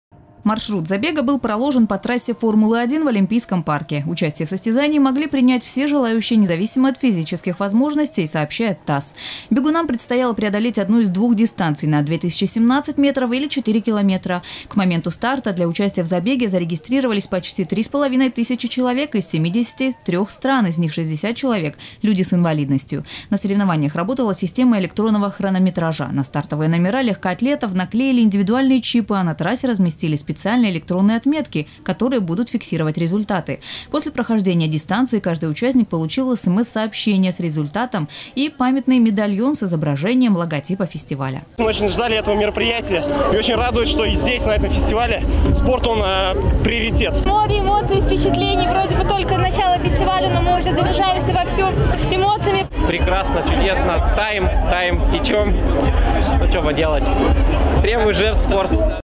Представители 73 стран приняли участие в забеге на 2017 метров на фестивале в Сочи. Новости Эфкате
На старт самого массового спортивного мероприятия в рамках Всемирного фестиваля молодёжи и студентов вышли около 3,5 тысяч человек.

Маршрут забега был проложен по трассе «Формулы-1» в Олимпийском парке. Участие в состязании могли принять всех желающие независимо от физических возможностей, сообщает ТАСС. Бегунам предстояло преодолеть одну из двух дистанций — на 2017 метров или 4 км. К моменту старта для участия в забеге зарегистрировались почти 3,5 тысячи человек из 73 стран, из них 60 человек — люди с инвалидностью.

На соревнованиях работала система электронного хронометража. На стартовые номера легкоатлетов наклеили индивидуальные чипы, а на трассе разместили специальные электронные отметки, которые будут фиксировать результаты. После прохождения дистанции каждый участник получил смс-сообщения с результатом и памятный медальон с изображением логотипа фестиваля.

Сочи. Новости.
Телеканал Эфкате.